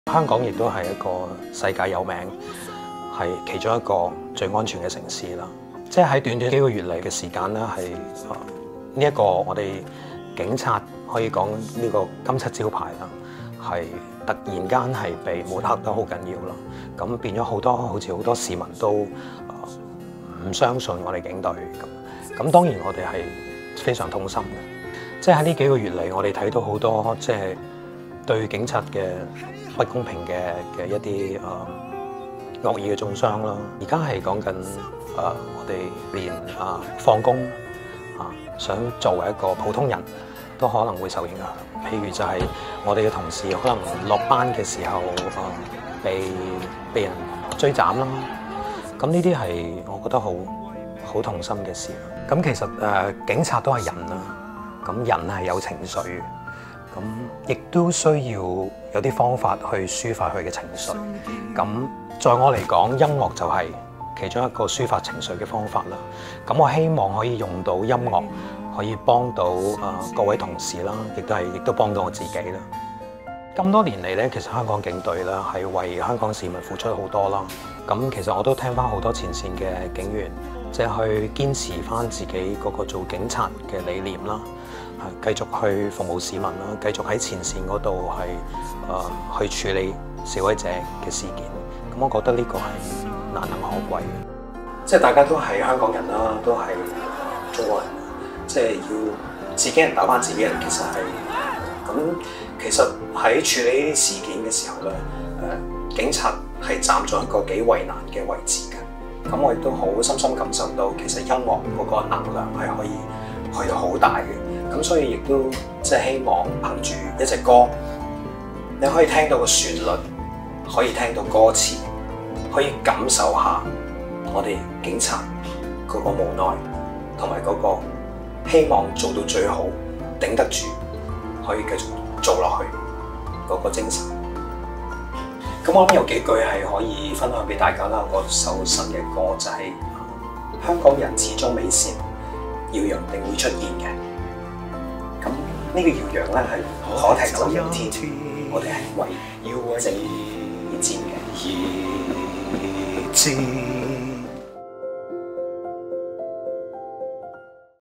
【憑歌寄意】警隊抵抗暴徒承受前所未有壓力！總督察譚威信憑歌抒情寄意︰香港警隊為市民付出很多、希望音樂可幫到同事
//眼見出生入死嘅好兄弟，係呢幾個月因工作而心情低落，譚sir當然好心痛，所以自彈自唱，希望用音樂紓緩大家情緒，互相鼓勵//